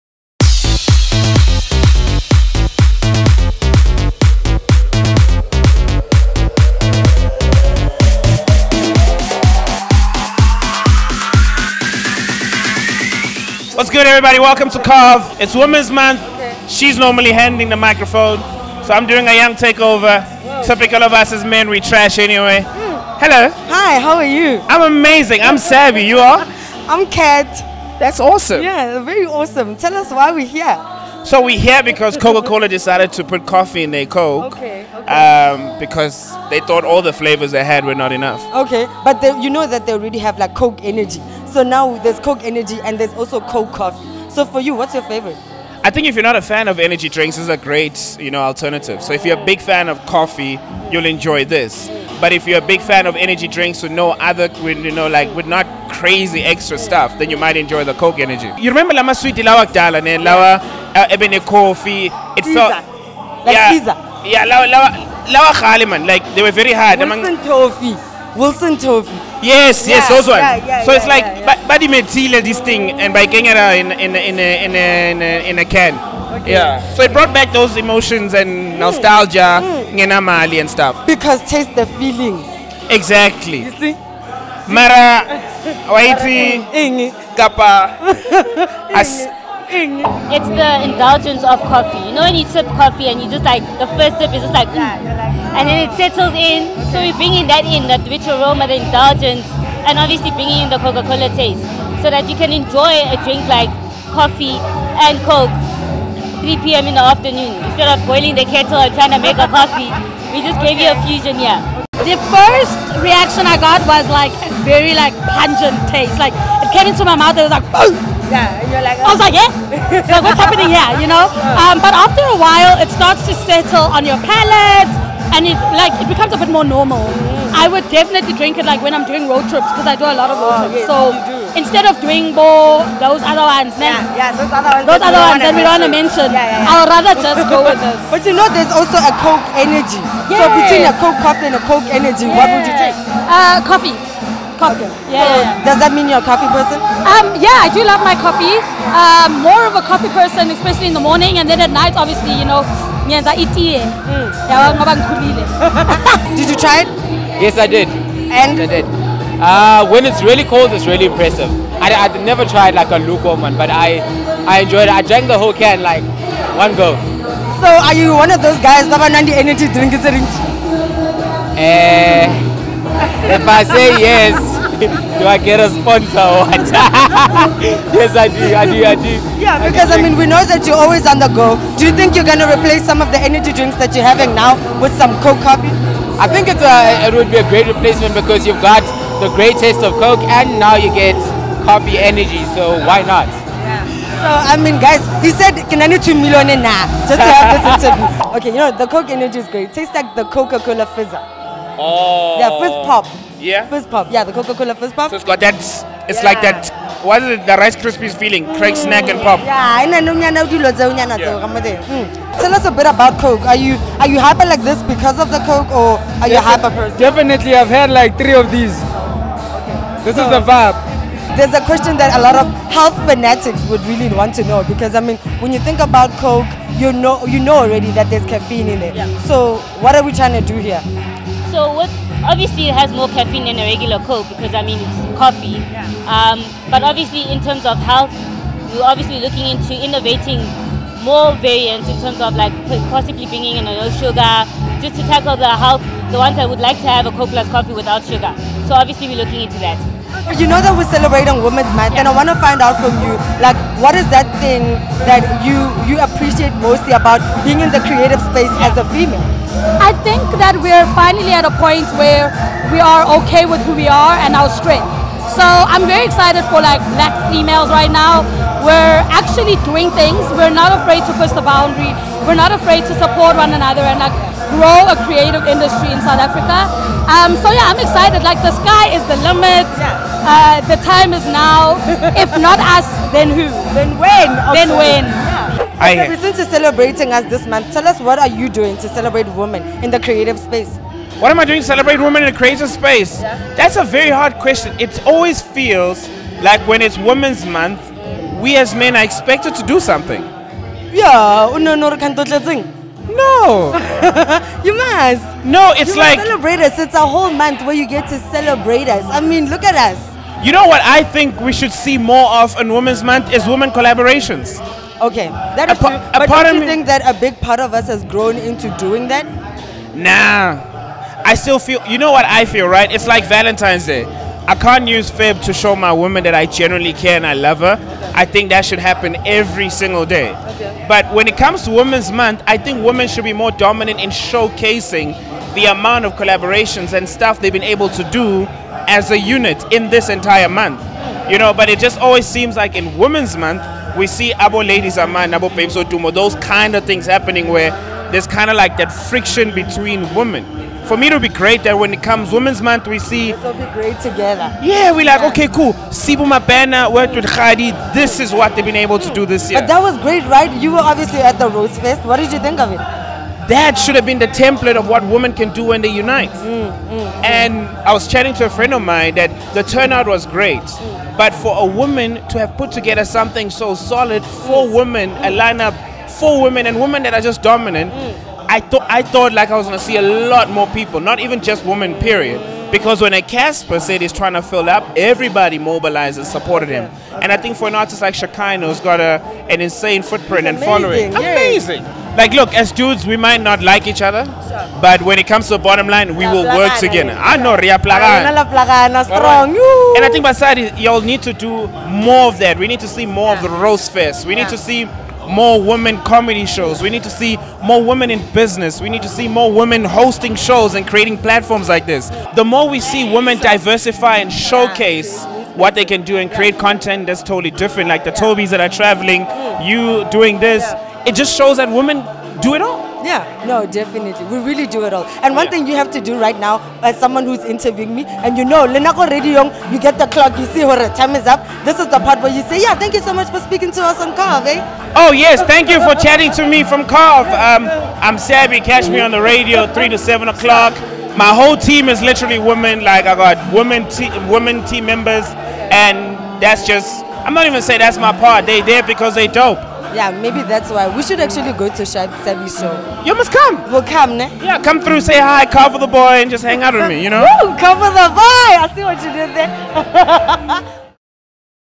Coca-Cola Plus Coffee Launch
Having a Coca-Cola nowadays is more than tasting the feeling of what we are used to. Coca-Cola South Africa has recently launched two new variants to the family. Gone are the days when it was just Vanilla Coke; this brand now has an energy drink and surprisingly another variant that packs on more caffeine. We attended the Coca-Cola Plus Coffee launch to unpack some of the questions that everyone might have about this beverage. We caught up with the whose who of the creative industry to find out what they rate it

Shot by: @DartPhotography
Edited by: @onemocut
Produced and presented by: @selepe_m